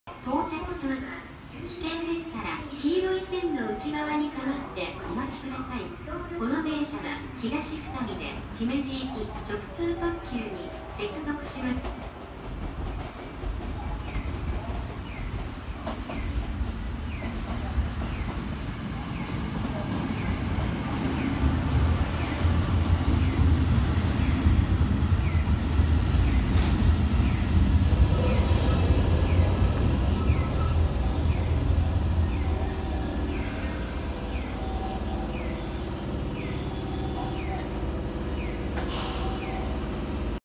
［偽アルミ］山陽3100形(3100F)普通姫路行き 山陽明石駅入線
15時37分入線
山陽3100形3100Fは3100・3101はアルミ、3619は鋼製です。